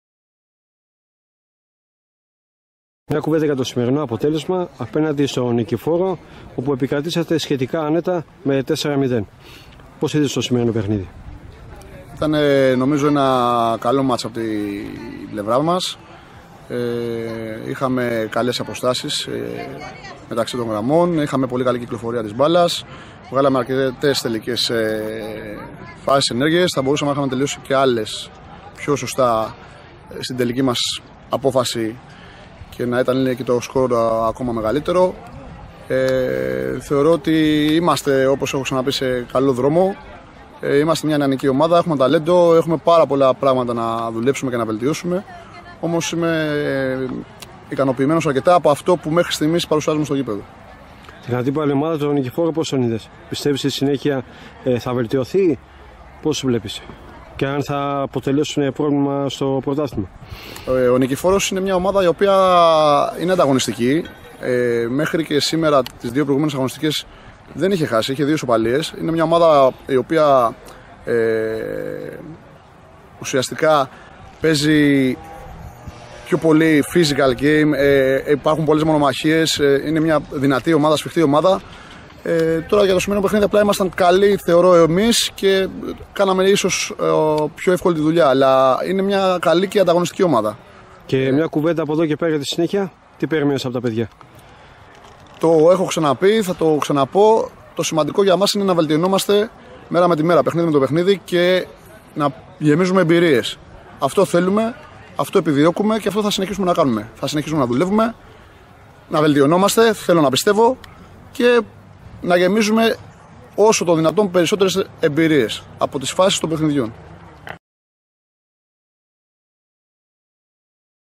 Αστέρας Βάρης Β' - Νικηφόρος 4-0 |δήλωση Καβάσης Γ. Γ' ΕΠΣΑΝΑ - 9/10/2021| vivasports
Αστέρας Βάρης Β' - Νικηφόρος 4-0
δήλωση Καβάσης Γ. Γ' ΕΠΣΑΝΑ - 9/10/2021| vivasports

📷   Γιώργος Τσιφλίκας   📹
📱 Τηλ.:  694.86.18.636